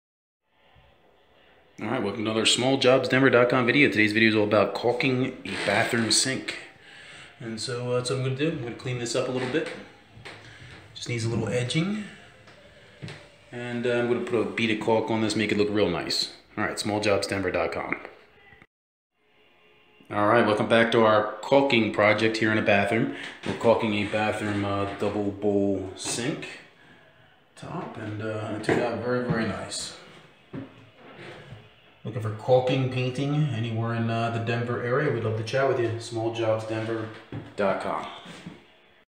Denver Handyman: Bathroom Countertop Vanity Caulking
There are many types of caulk for both interior and exterior applications.  In this video we are caulking a double bowl bathroom sink counter top.  This small handyman project really finishes this bathroom painting project.   Nothing makes a painting job look better than caulking and sealing all the cracks and seams.